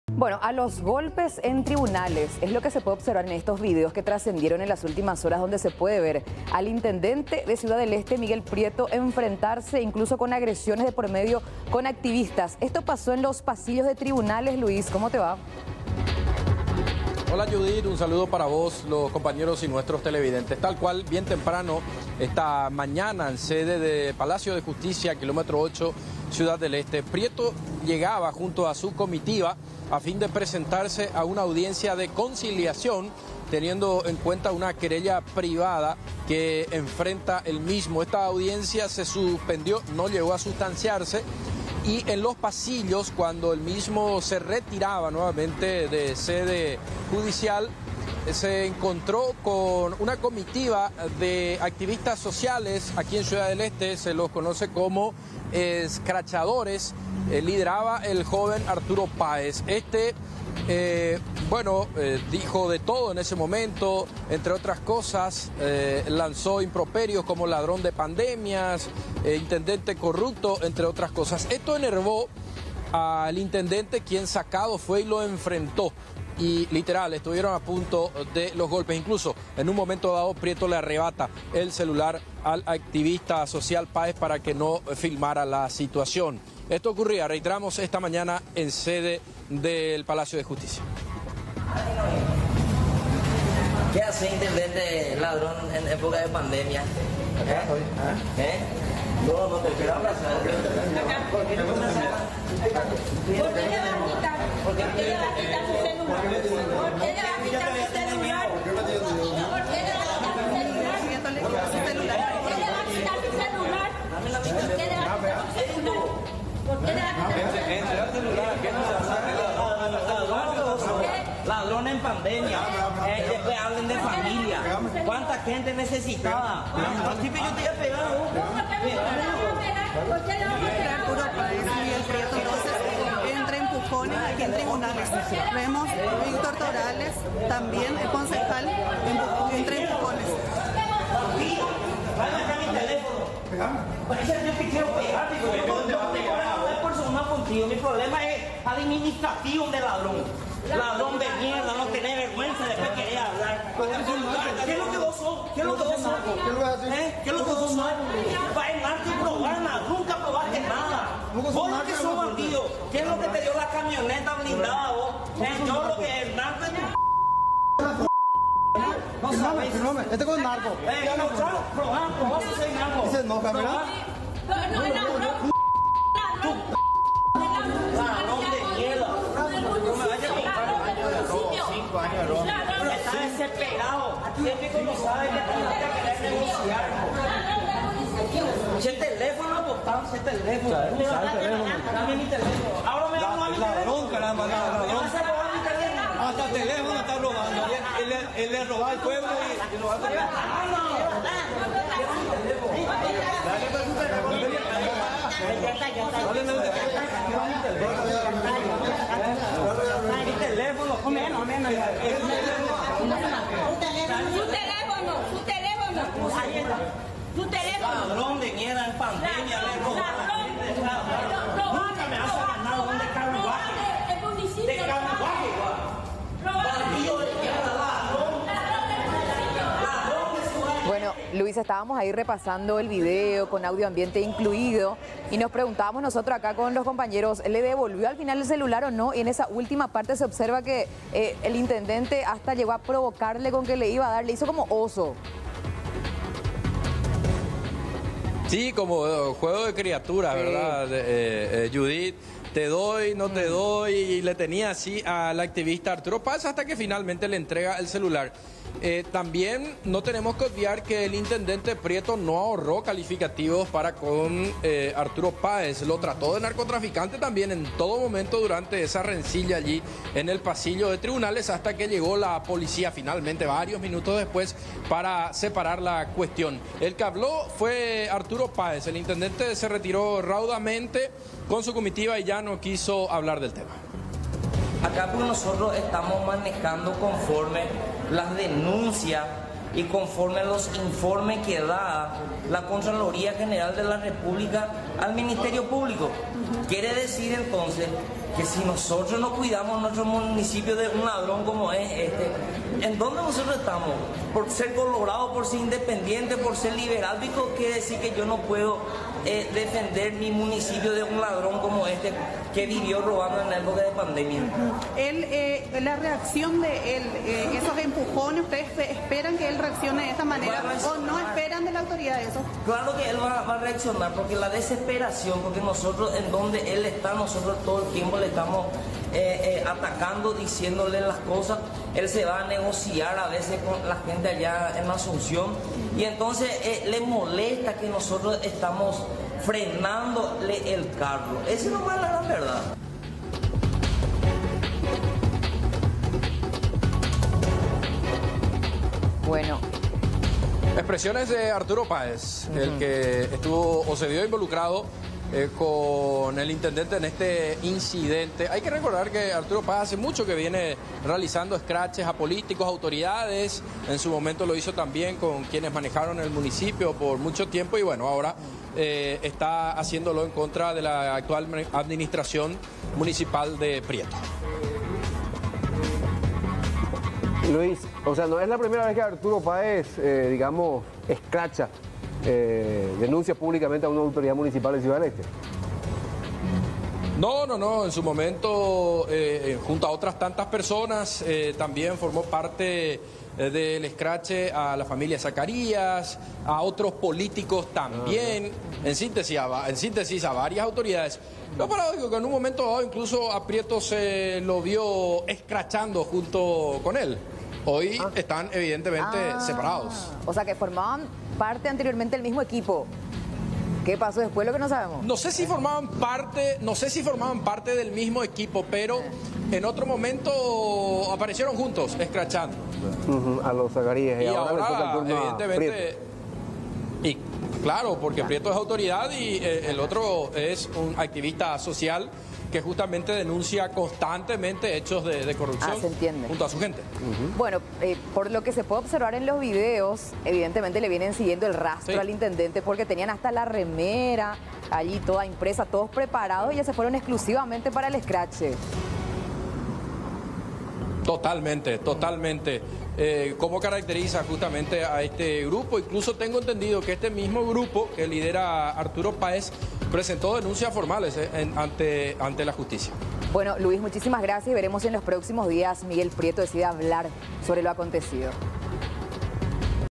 Intendente de CDE fue escrachado durante una audiencia y reaccionó a los golpes
Miguel Prieto, intendente de Ciudad del Este, fue escrachado por un grupo de ciudadanos tras llegar a Tribunales para enfrentar una audiencia y reaccionó golpeando a uno de los hombres que lo estaba filmando e insultando.